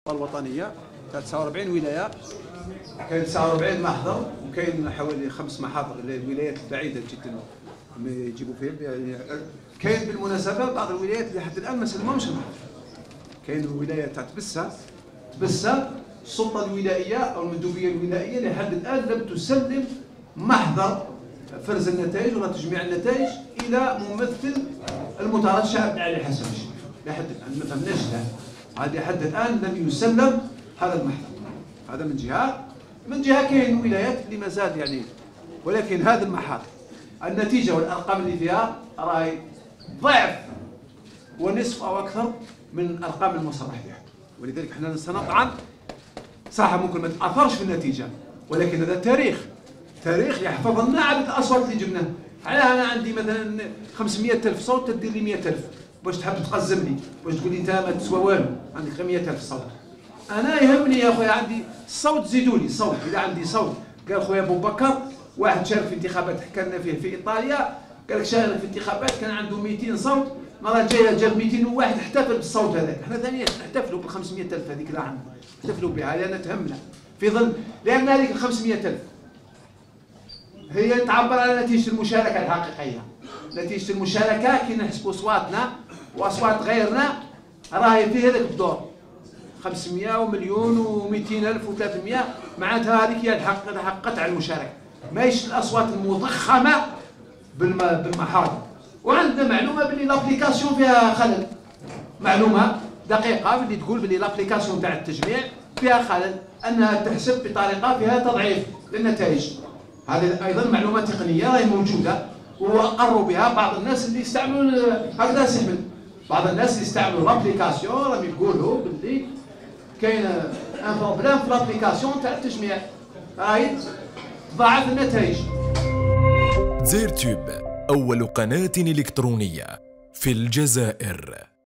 .المحاضر الولائية ماشي كيما النهائية .. حبوا يقزمونا و التطبيق تاع السلطة فيه خلل
شاهد..عبد العالي حساني معلقا على محاضر فرز الأصوات...المحاضر الولائية ماشي كيما النهائية .. حبوا يقزمونا و التطبيق تاع السلطة فيه خلل